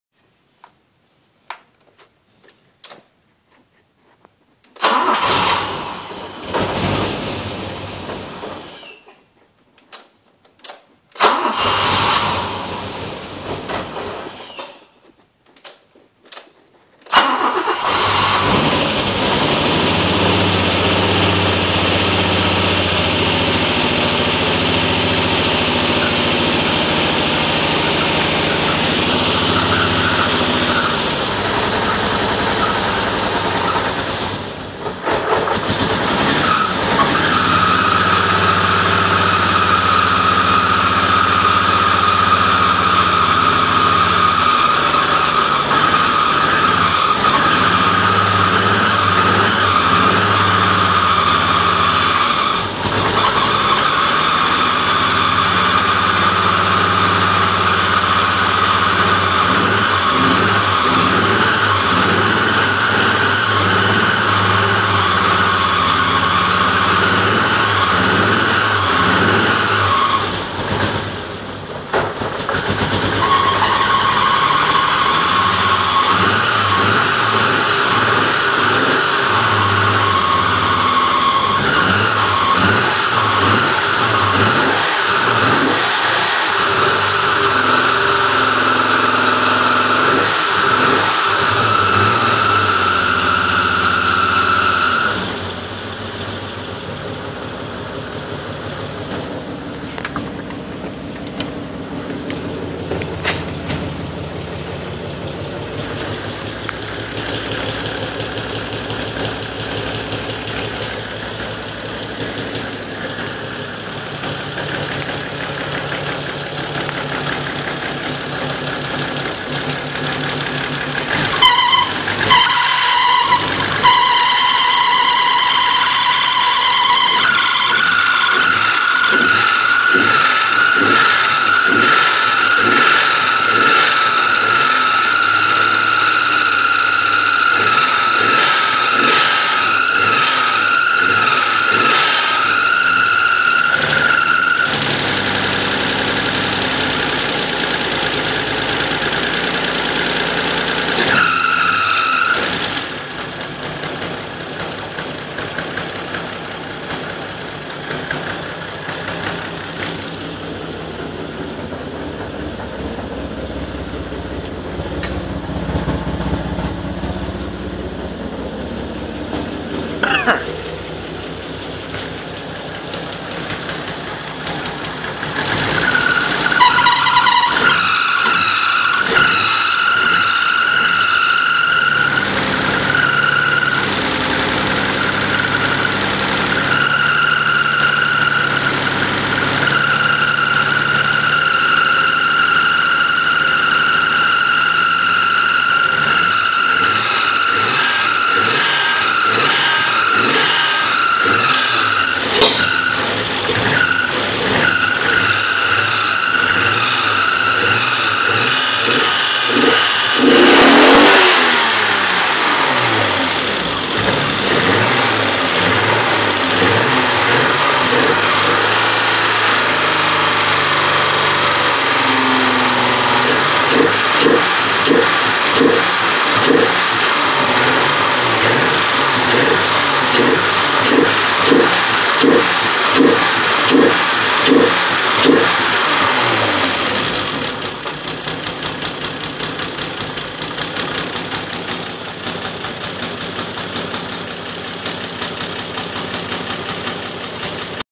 Alfa Romeo 3.0 first startup
First start in about 5 years of my Alfa 75 3.0 America -90!
Still some work to make it run smoothly tho..
(Don't mind the squaring belt)